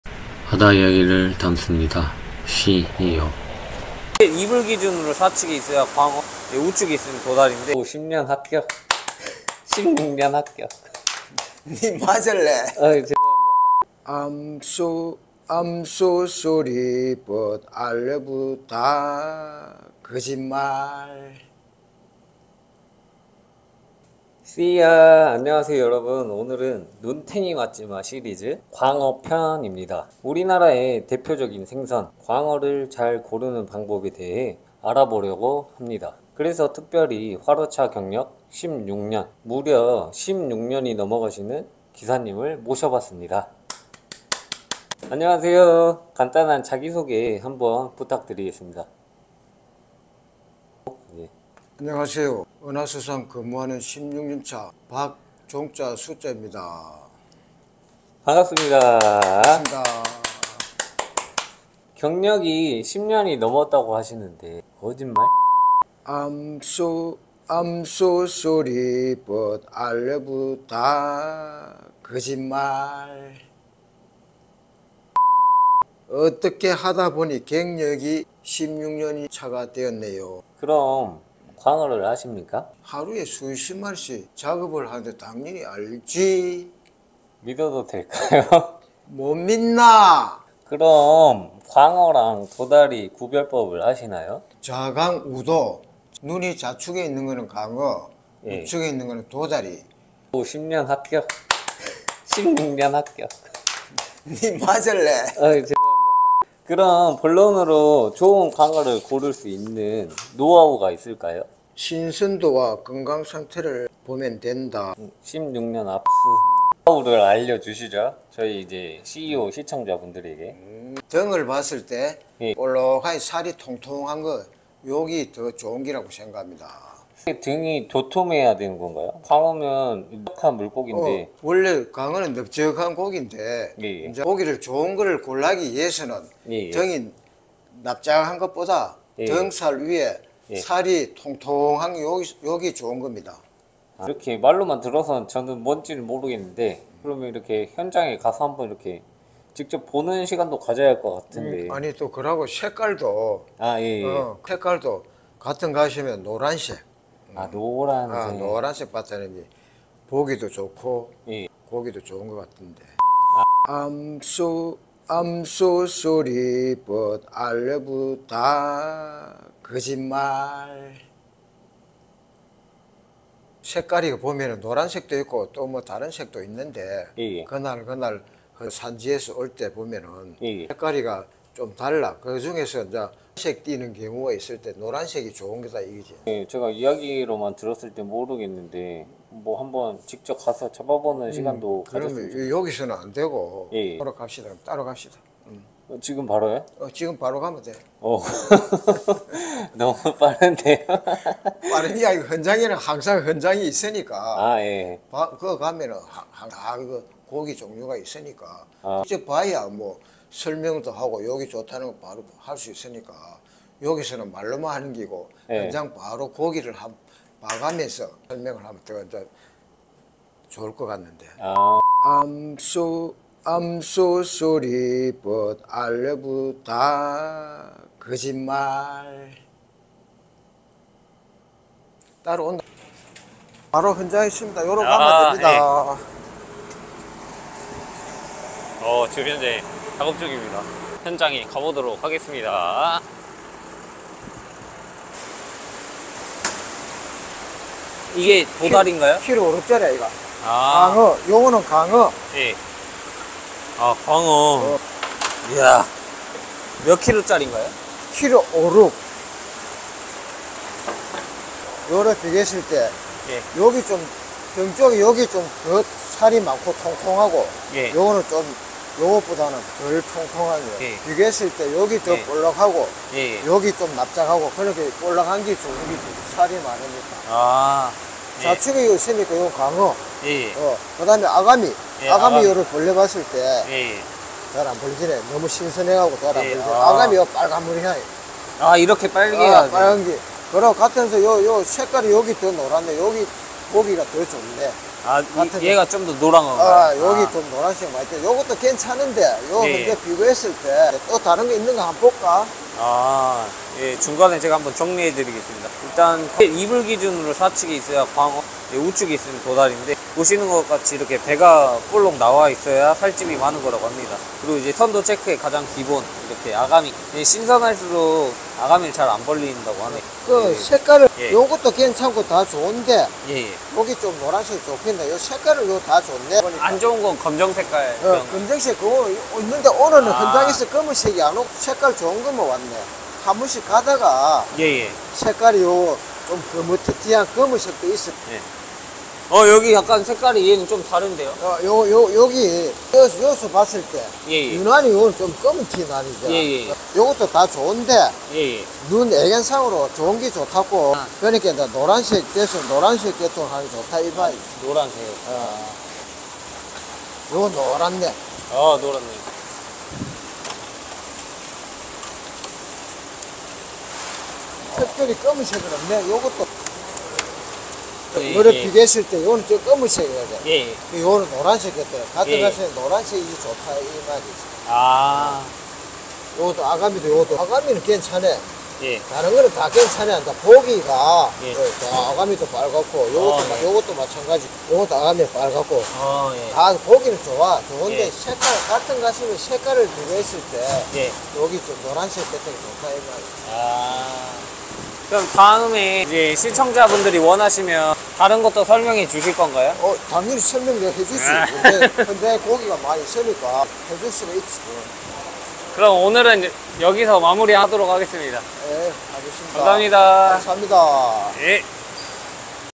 수산물 시장 눈탱이 무섭지 않나요?? 16년차 경력자가 알려주는 광어 고르는 방법
바다이야기를 담습니다.

안녕하세요 SEA EO입니다!

오늘은 많은 분들이 좋은 수산물을 고르길 바라면서 업로드 해봅니다!
국민 생선 횟감 광어 다들 아시죠??

많이들 드시고 계시지만 고르는 방법을 잘 모르시겠다구요??
이 영상으로 종결해봅니다 하하하하

좋은 자료가 되길 바랍니다! 감사합니다!